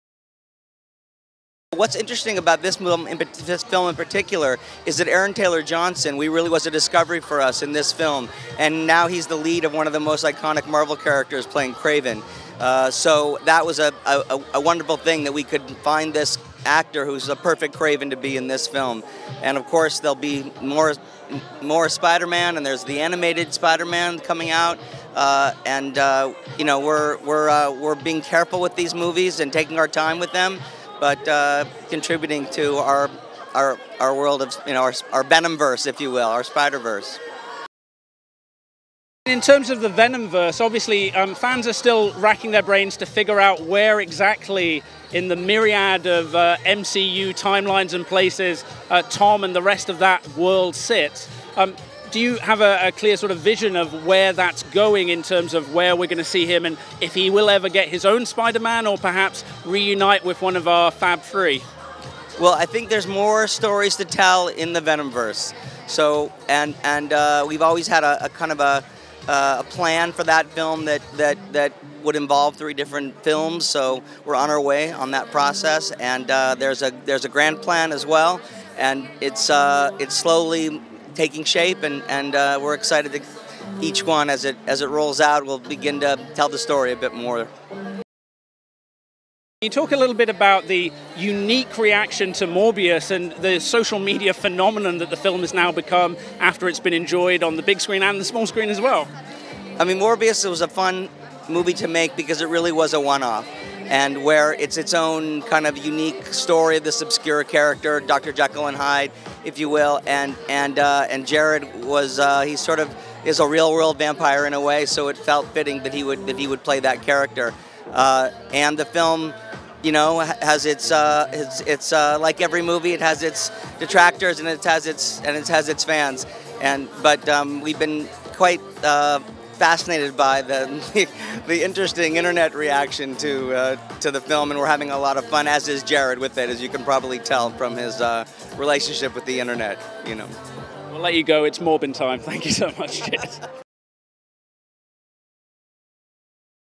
Sony Pictures President Sanford Panitch On Spiderverse Master Plans
Red Carpet News TV provides coverage from the Bullet Train UK Gala Premiere In London’s Leicester Square. The amazing new film is directed by action genre visionary David Leitch and boasts an all-star cast lead by Brad Pitt.

Red Carpet interviews include Brad Pitt, Aaron Taylor-Johnson, Brian Tyree Henry, Joey King, Producer Kelly McCormick, Director David Leitch and Sony Pictures Motion Picture Group President Sandford Panitch.

Additional VIP guest interviews include Made In Chelsea star Sophie Hermann alongside Love Island 2022 contestants Coco Lodge, Chyna Mills & Josh Le Grove.